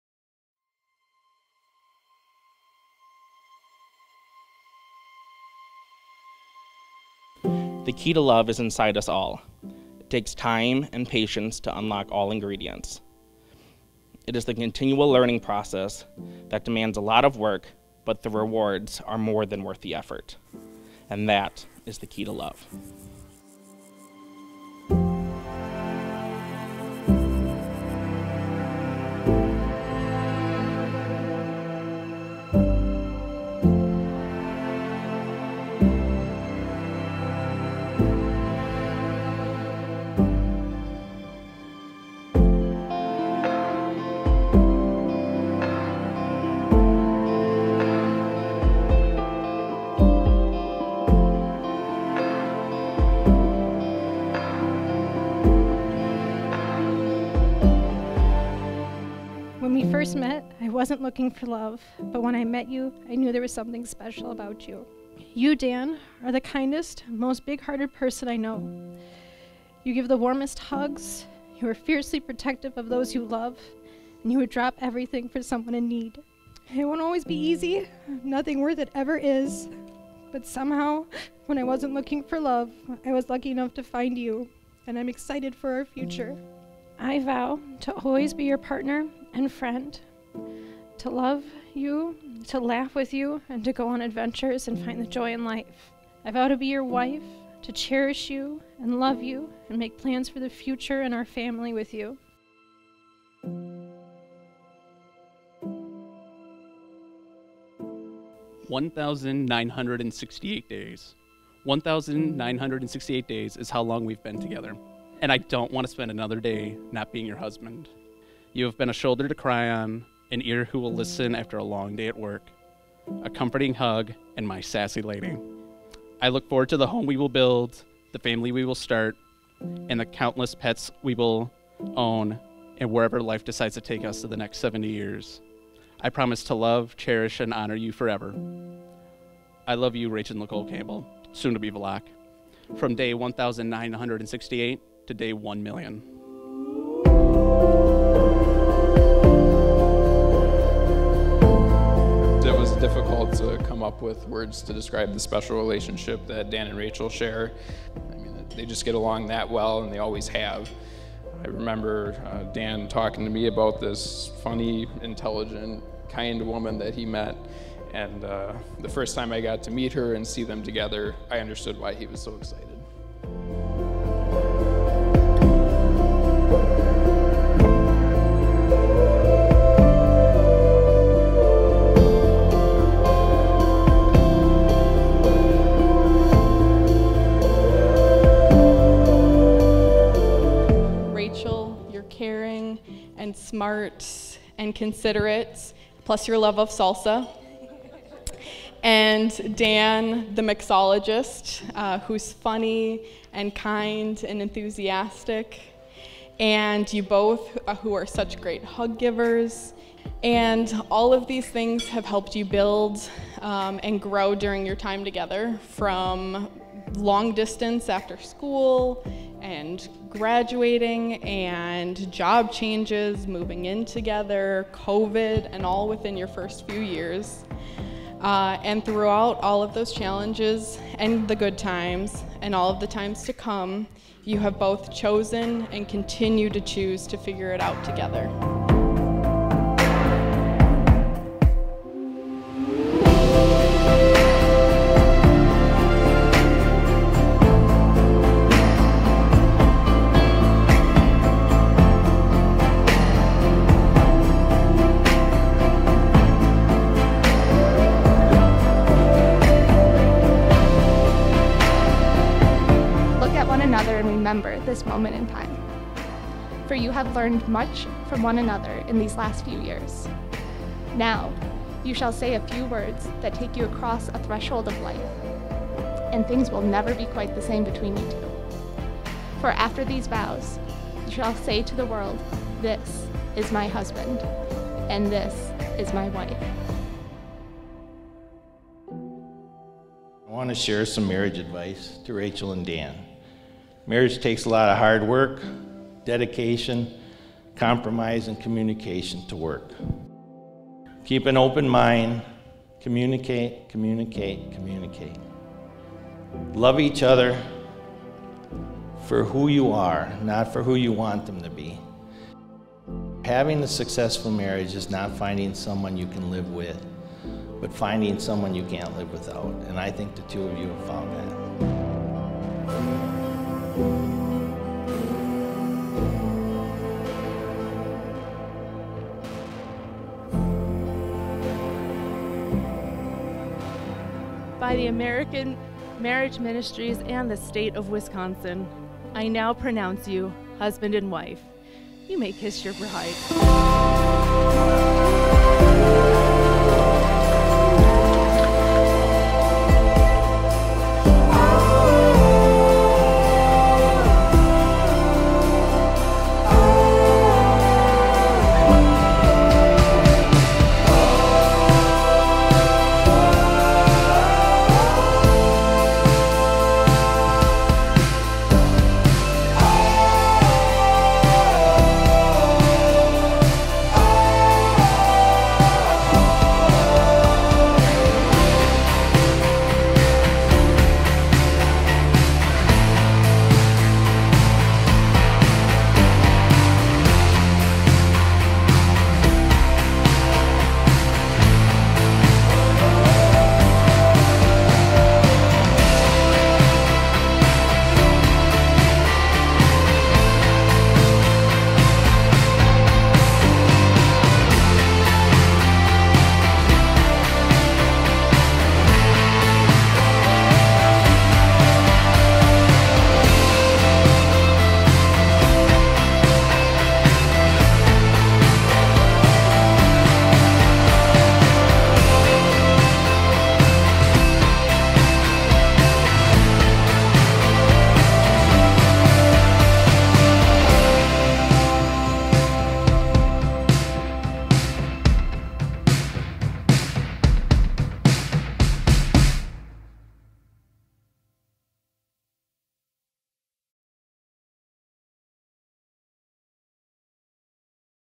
Rachel & Dan | Wedding Film | 8-27-22
This day was perfect! Had to steer clear of rain and use a greenhouse. It was so pretty! Thank you so much for trusting me to capture your day. Enjoy this timless keepsake!

Main Camera
Main Lens
B Camera
C Camera

** Wedding Team **
-Videographer: Frassetto Films
-Photographer: GreatScott Images
-Venue: Thornberry Creek at Oneida
-DJ: Record Entertainment Disc Jockey Service & Photobooths
-Floral: Nature's Best Floral & Boutique